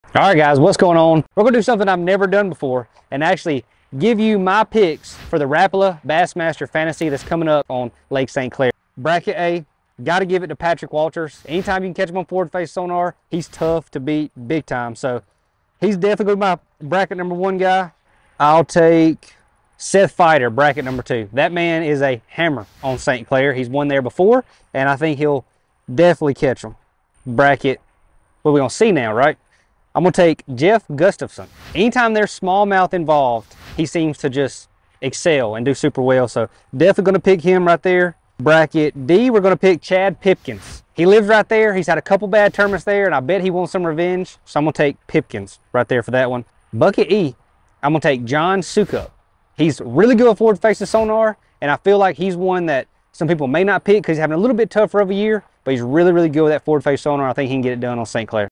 Kyle Welcher picks his St. Clair Bassmaster Fantasy Fishing team
Bassmaster Angler of the Year leader @KyleWelcher gave his take on Rapala Bassmaster Fantasy Fishing for Lake St. Clair. He picked his 5 angler lineup and you can too, go to BassmasterFantasy.com to sign up for free and set your team.

Who will win this week?

Kyle’s team:
Bucket A - Patrick Walters
Bucket B - Seth Feider
Bucket C - Jeff Gustafson
Bucket D - Chad Pipkens
Bucket E - John Soukup